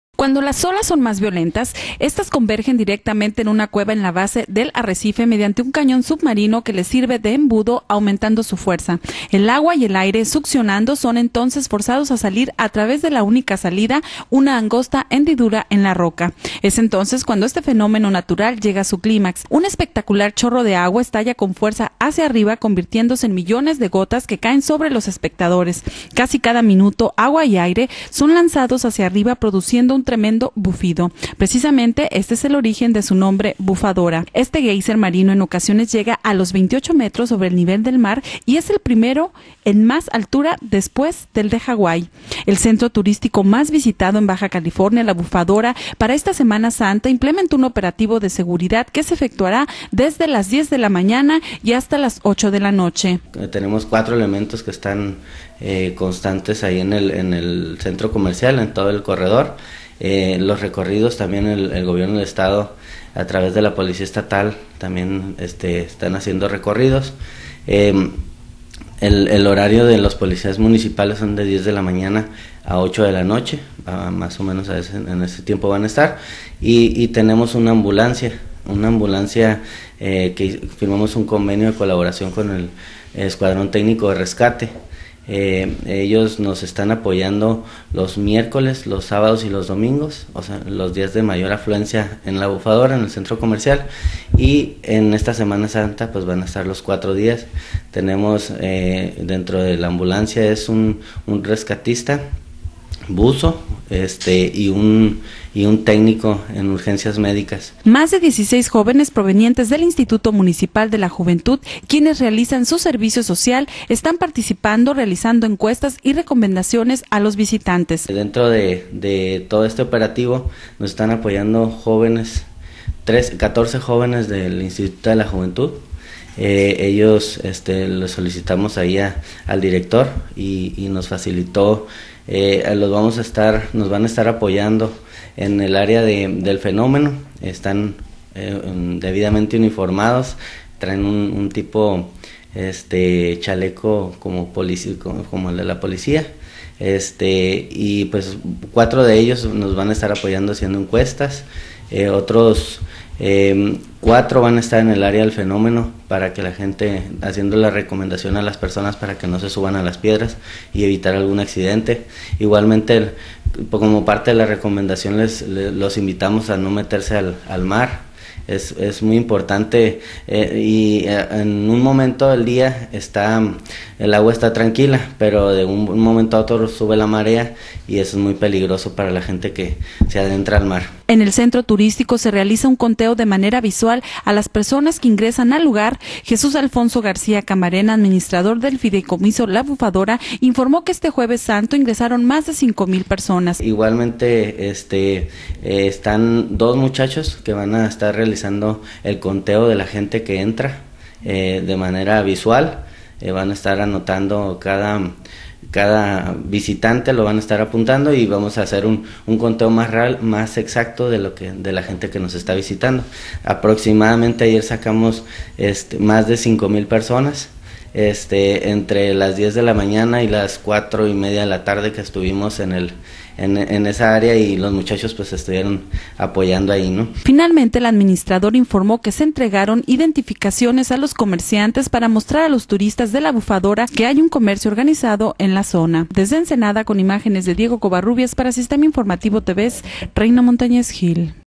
La Bufadora Enseanda Baja California
En entrevista con el líder de locatarios de La Bufadora en Ensenada, Baja California, explica las mejoras, los atractivos y el operativo de Semana Santa 2012